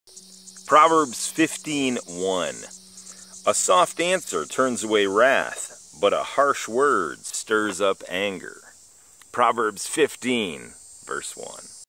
Proverbs 15:1 ESV "A soft answer turns away wrath, but a harsh word stirs up anger."
Tom reads Proverbs 15:1 ESV
"A soft answer turns away wrath, but a harsh word stirs up anger."

Proverbs 15:1 ESV